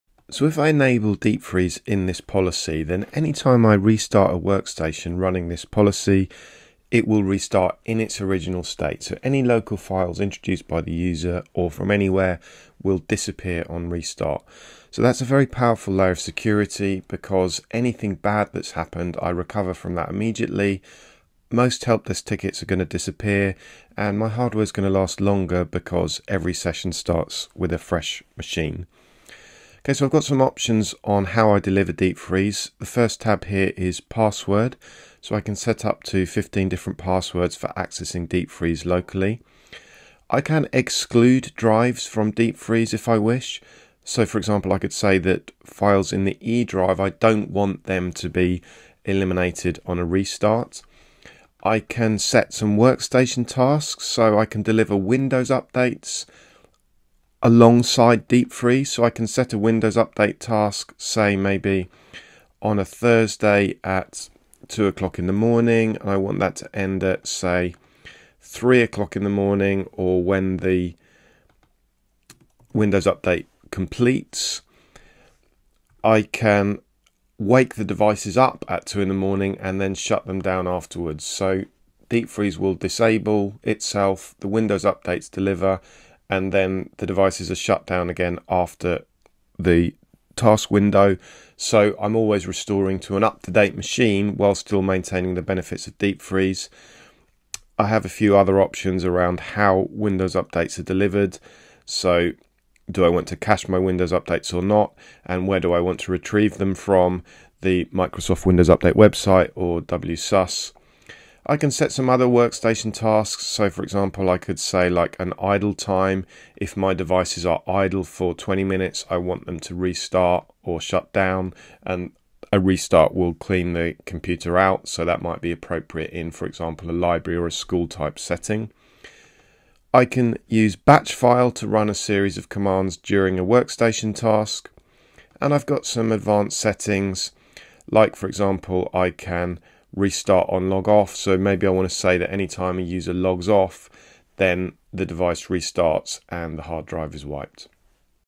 Faronics Deep Freeze Demo
See Deep Freeze in action! This quick demo shows how you can protect your endpoints and restore them to a perfect state with every reboot. Say goodbye to unwanted changes, malware, and downtime.

✅ Prevent configuration drift
✅ Instantly undo system changes
✅ Reduce IT support costs